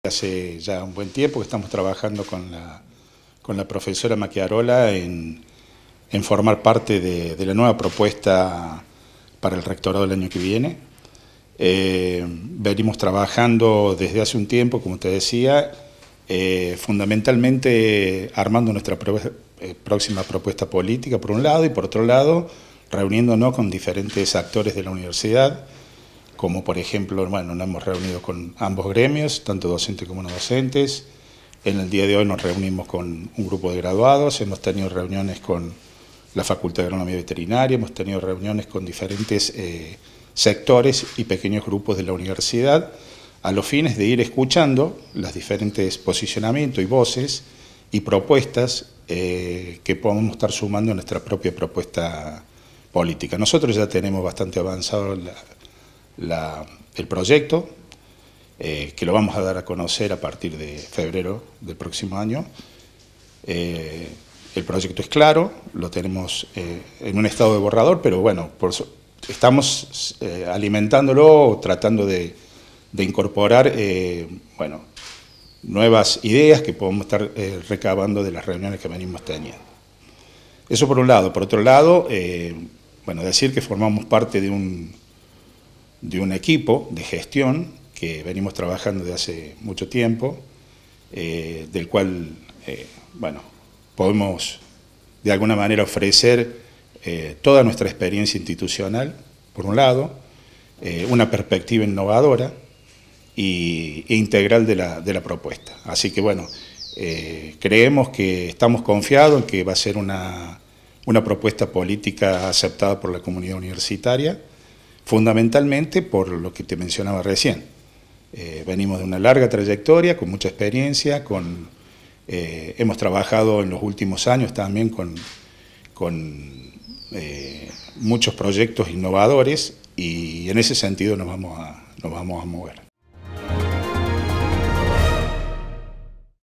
ELECCIONES RECTORALES 2023: CONFIRMARON LA PRESENTACIÓN DE LA FÓRMULA GONZÁLEZ - MACCHIAROLA
SERGIO GONZÁLEZ - CANDIDATO A RECTOR